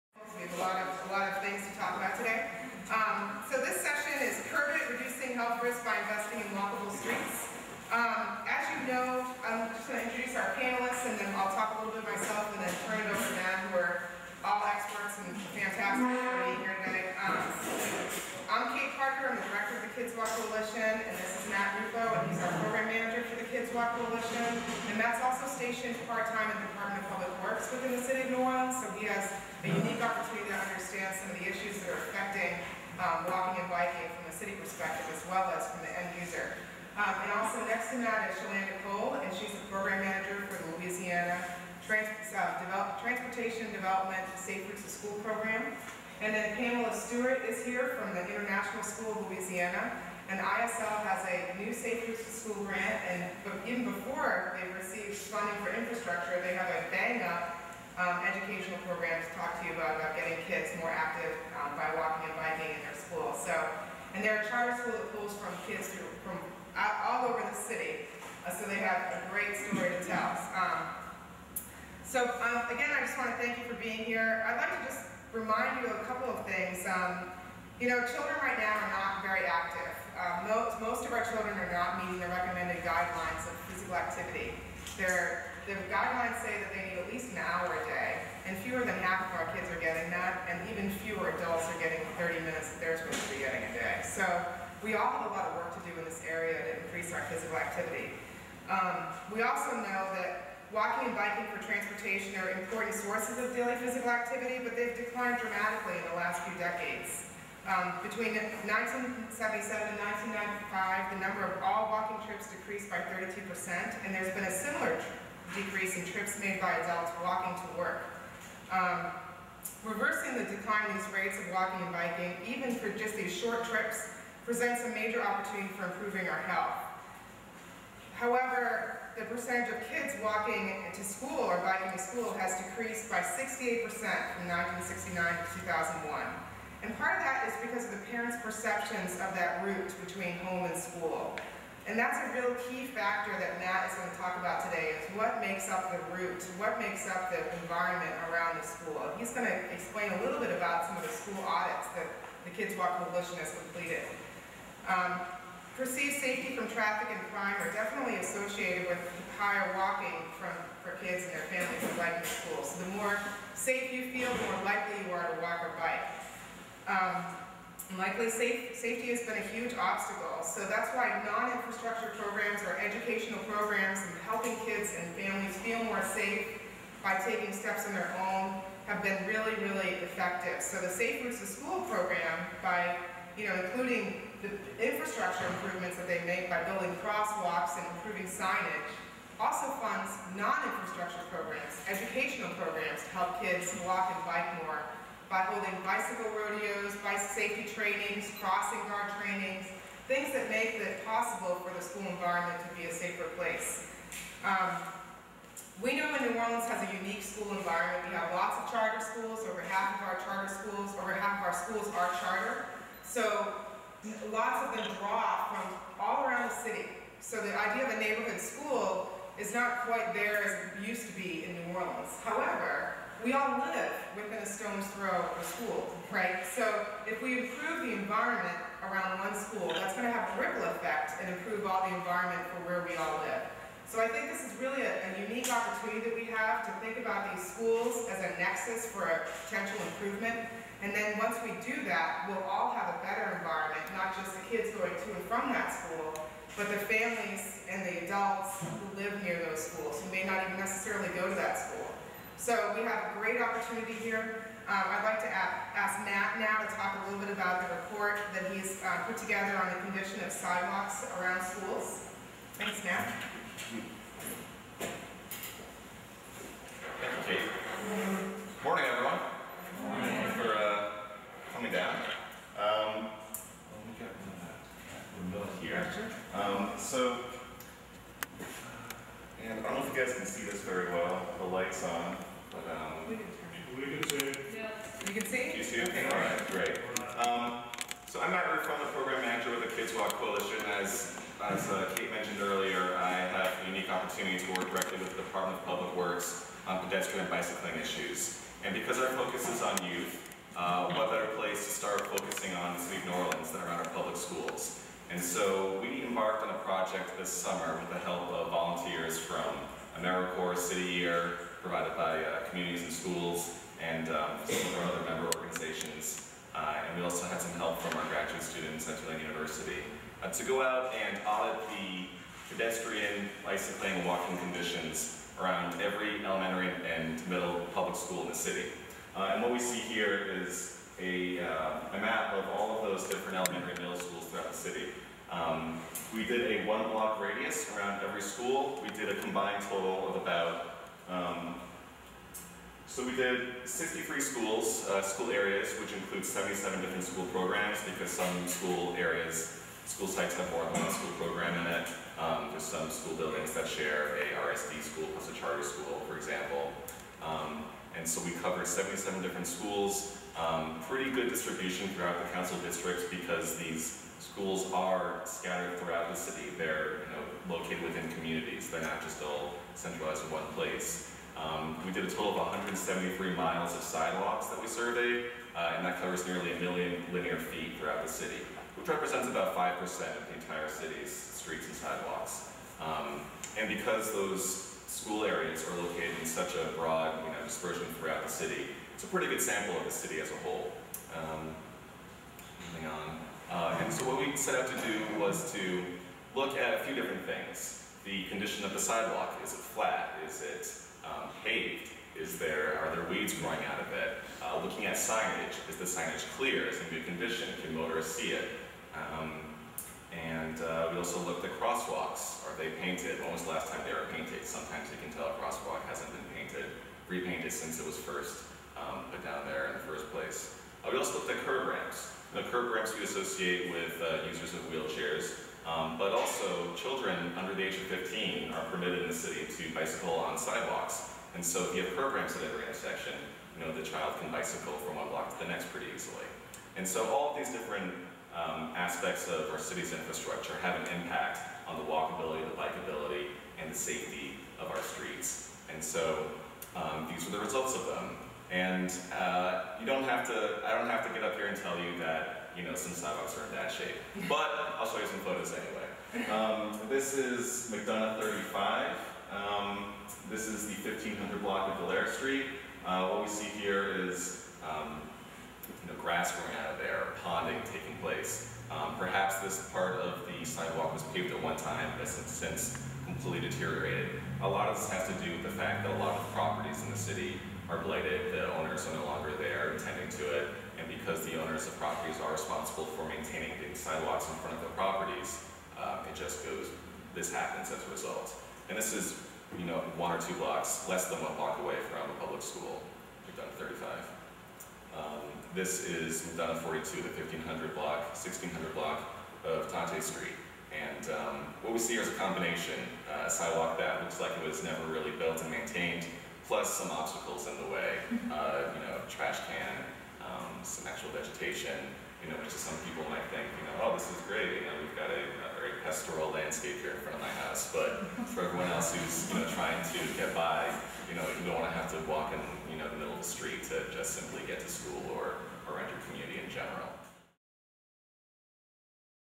Curb It! - Walk and Roll Louisiana Summit 2010 - Part 1
Walking, bicycling and being physically active in other ways means reducing risk for obesity and the health problems it causes, such as heart disease and diabetes.  Research shows that children will be active if given a safe, dedicated space to do so  This panel will review the health benefits of youth walking and bicycling, report on sidewalk and crosswalk conditions surrounding New Orleans public schools, offer information on Safe Routes to School grants, and give examples of what local schools are already doing to encourage their students to be more active.

Speakers include:
Kate Parker-Karst, Prevention Research Center at Tulane University
Shalanda Cole, LaDOTD Safe Routes to SChool Program
Matthew Rufo, Prevention Research Center at Tulane University
Pamela Stewart, International School of Louisiana